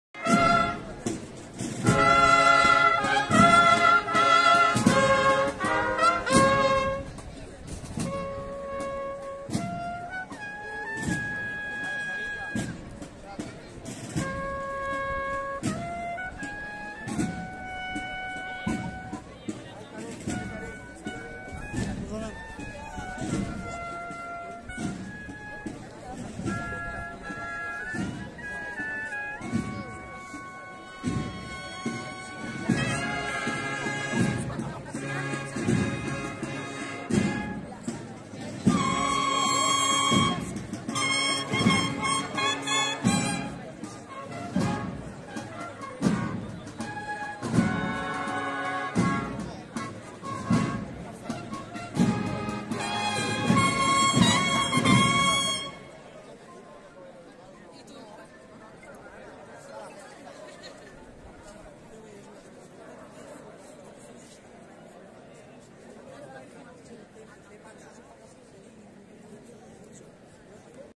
Domingo de Ramos en La Bañeza 2024
Comienza la Semana Santa en La Bañeza con los actos del Domingo de Ramos